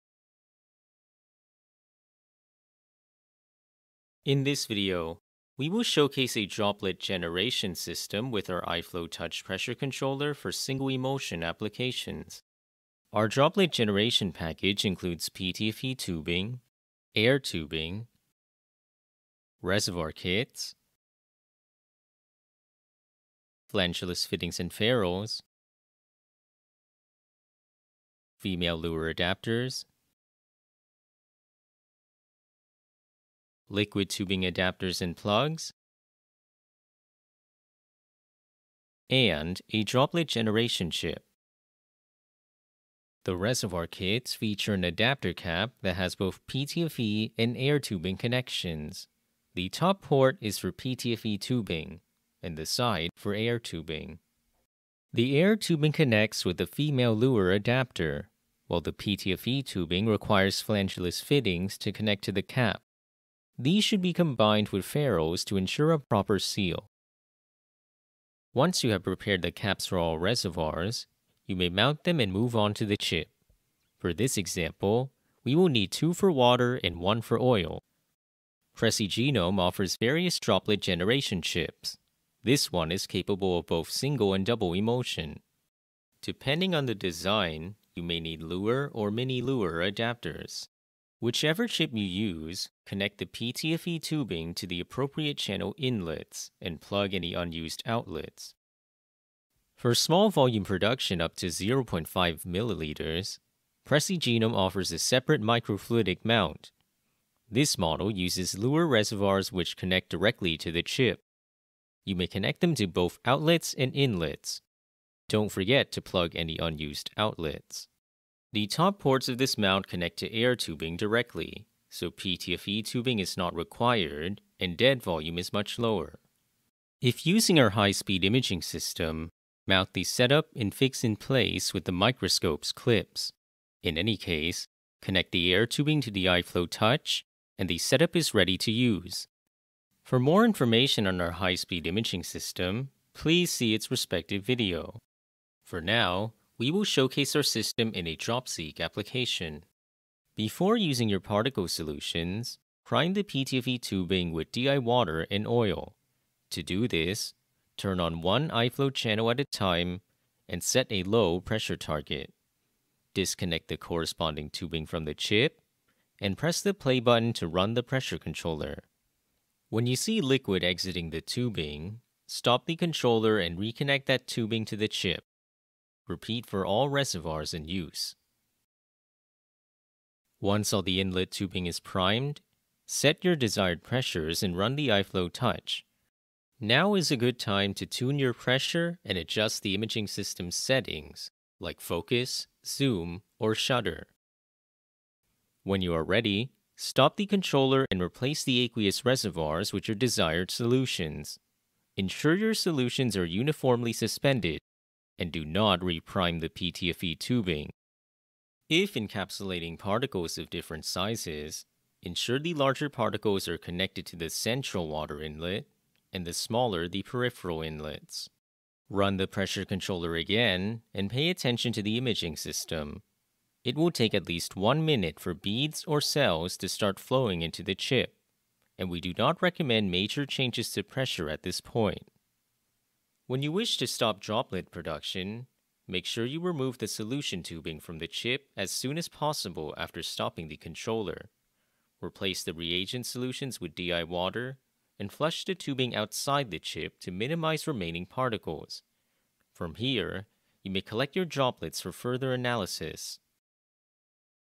iFlow Droplet Generator, single emulsion demo for single cell isolation, drop seq, digital PCR etc.
Droplet generator is the tool for generating micro/sub-micron sized droplets with highly reproducible capacity. PreciGenome Microfluidic Droplet Generation System PG-DG with droplet generator chips can be used for the rapid preparation of water-in-oil or oil-in-water droplets.

The benefits of iFlow microfluidic droplet generator can be scaled up to higher throughput. Parallel droplet generators allow high throughput production of monodisperse droplets, fit for applications in the pharmaceuticals, biology, and food industries. With our system, more than 10 million droplets with a diameter of  around 50-120um can be generated using our controller within one hour by using our starter kit. Smaller  50um or larger droplets 120um are able to be generated with proper chips and reagents.